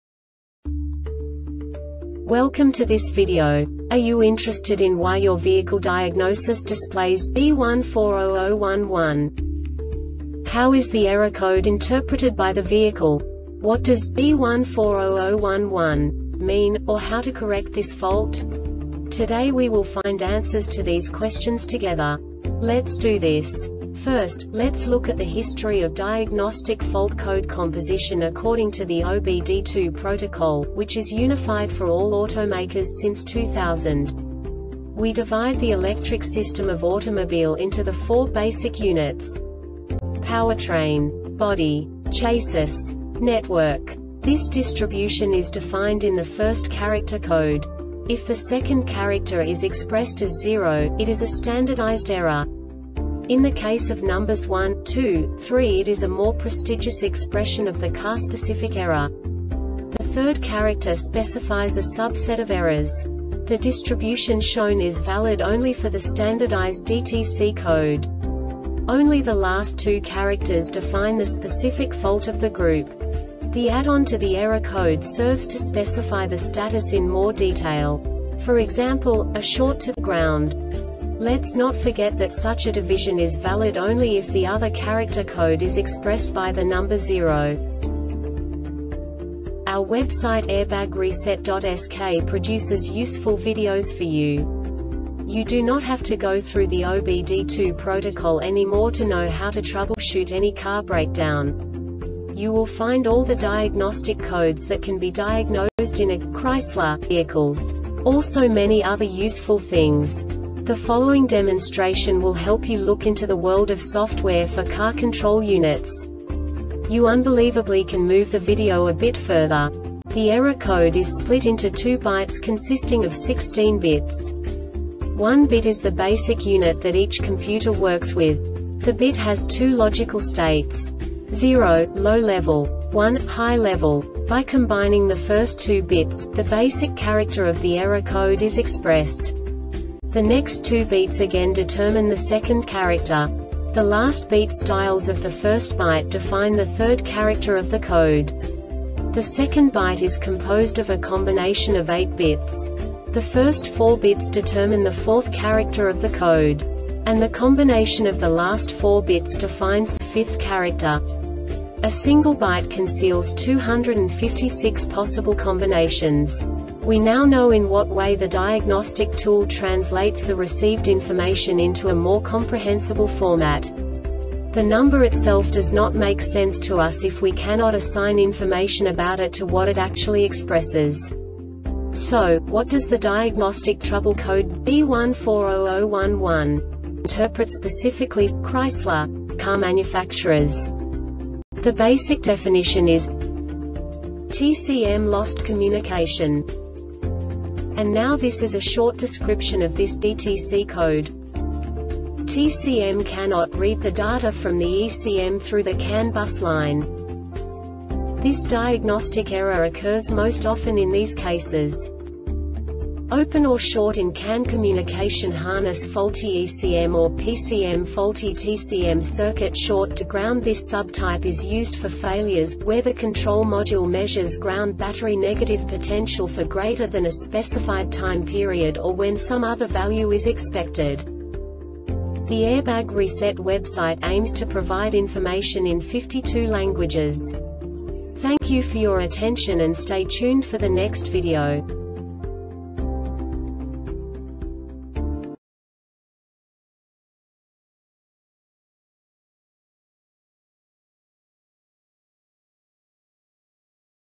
DTC Chrysler B1400-11 Short Explanation
Contents:
0:21 Basic DTC analysis according to OBD2 protocol standard.
1:48 Insight into programming
2:58 A diagnostically understandable description of the Diagnostic Trouble Code
3:05 Basic error definition

Make:
Chrysler
Code:
B1400 11
Definition:
TCM Lost Communication
Description: